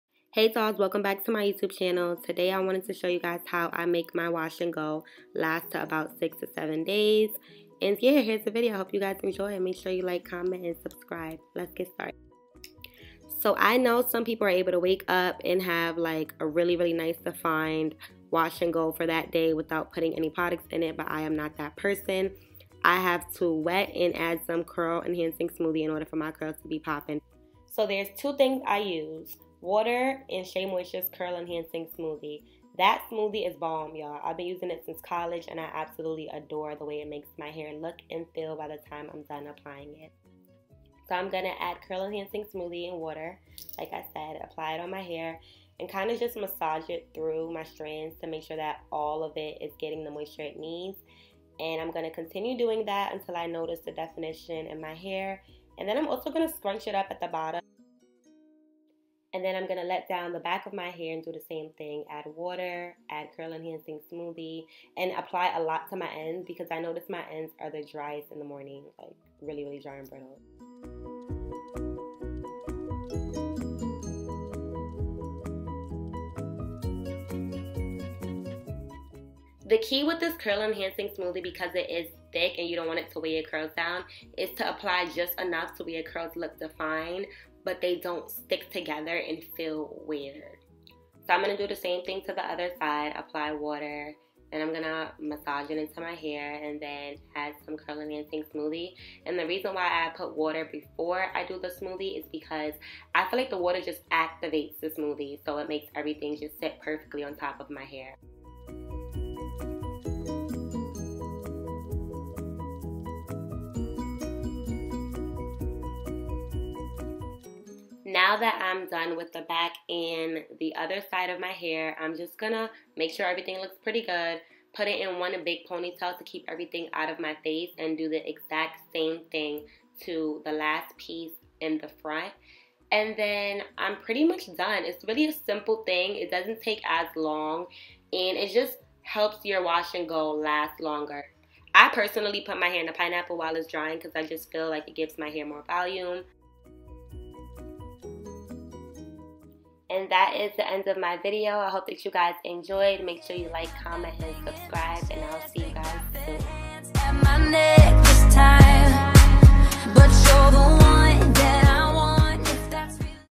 How to make your WASH & GO LAST UP TO 6 DAYS!
Hey Dolls, Here's my tutorial on how I make my Wash & Go last up to 6 days! I've been using Shea Moisture Curl Enhancing Smoothie for years now and I still love it. It is the only product I use to refresh my curls and it never fails. I love you guys. Sincerely Niya, XOXO.